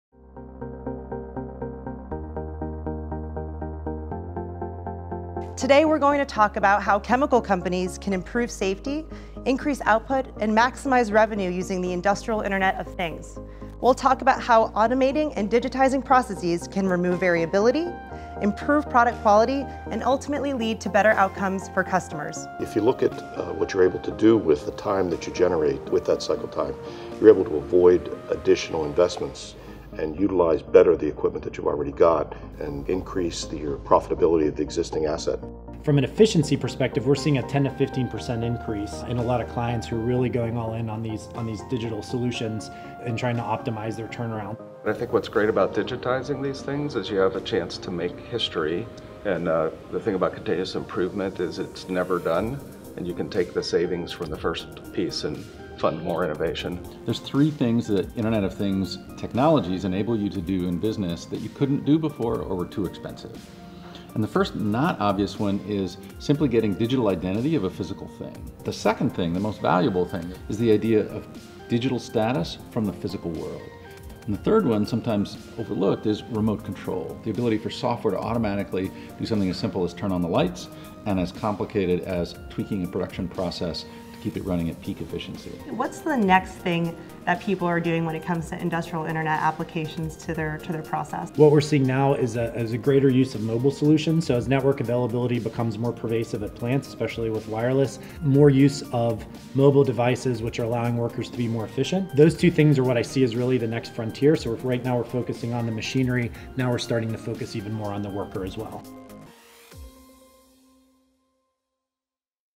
Unleash the Data: How Industrial Chemical Companies Can Maximize Revenue with IIoT
Pittsburgh-based PPG and GrayMatter co-innovated solutions to digitize key production processes and reduce costs.